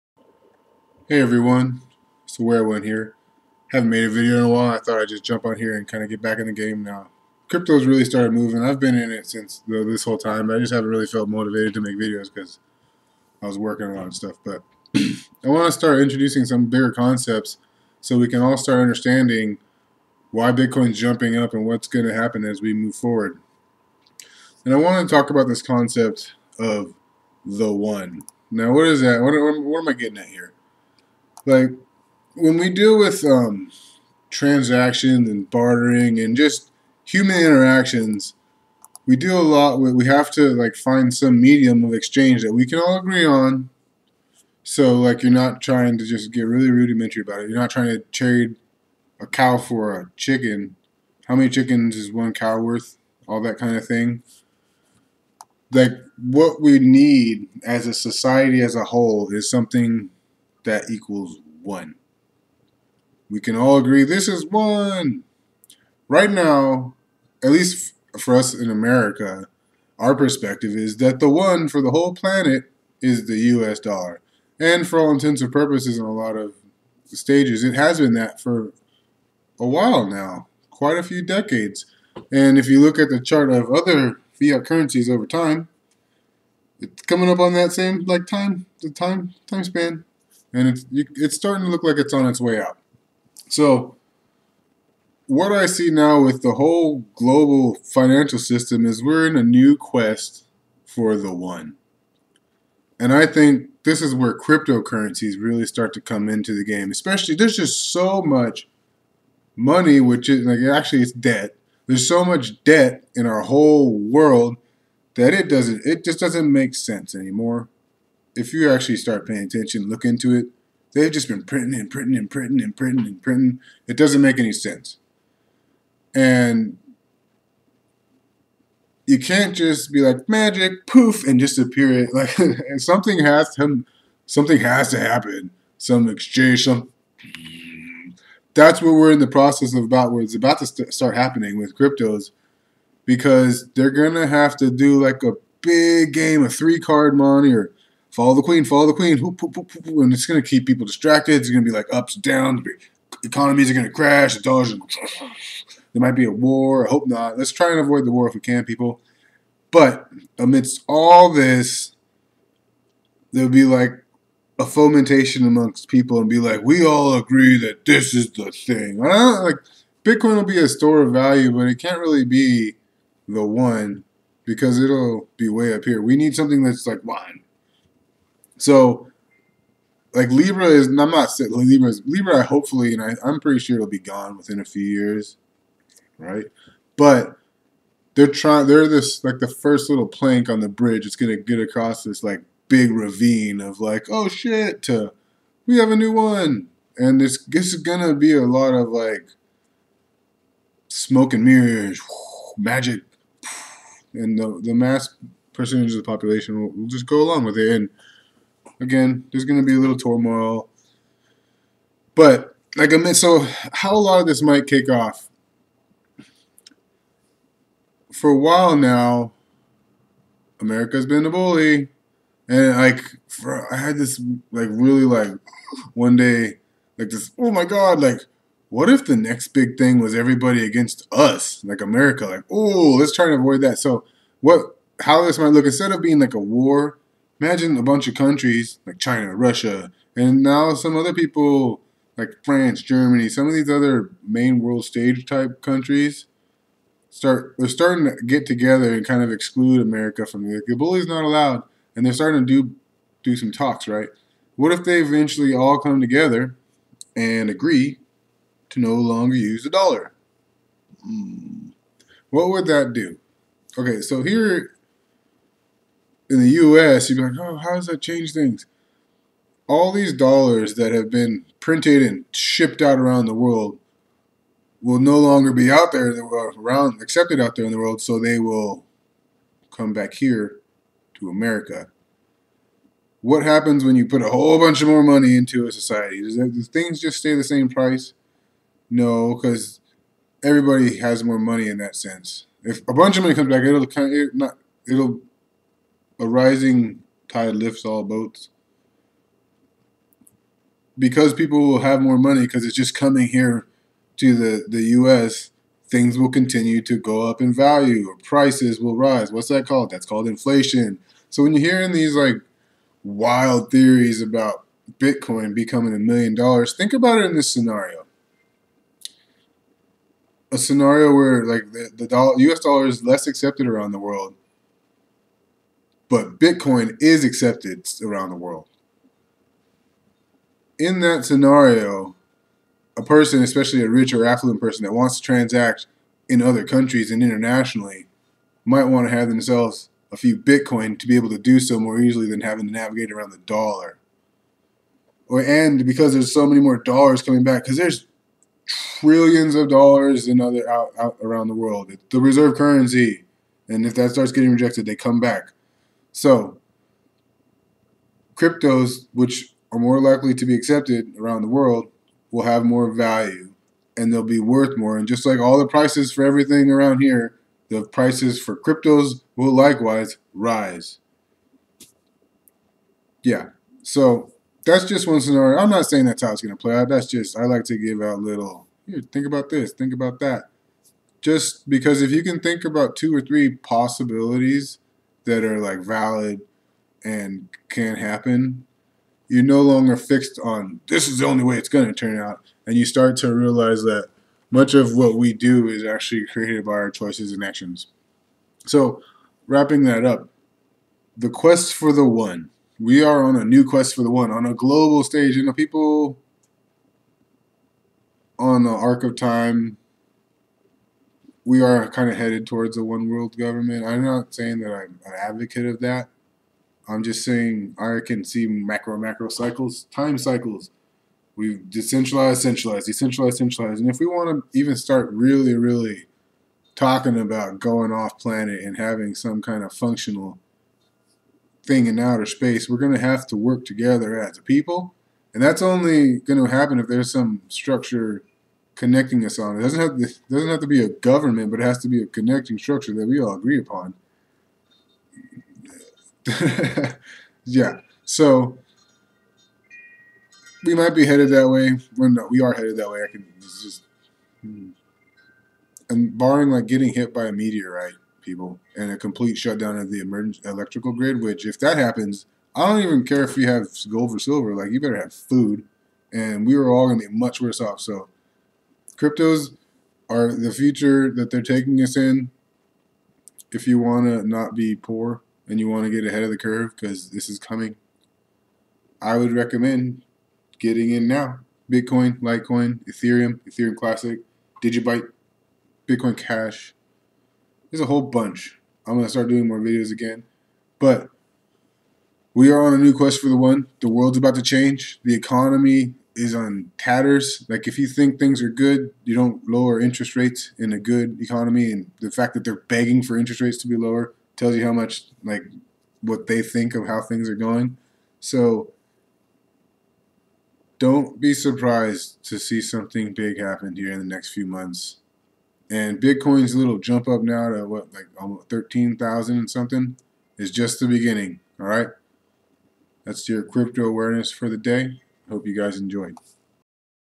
Cryptocurrencies and the Search For the 1
We are at a unique point in history where the world is in the process of establishing the new "1", the stable thing we all agree on to barter with. I believe cryptocurrencies are meant to play a big part in this process, and those who get in early will reap bigger benefits.

Once this really starts rolling out, it will be quick.

$BTC $ETH $BCH $NEO $ZRX $ELA $DGB $LTC $XTZ $ONT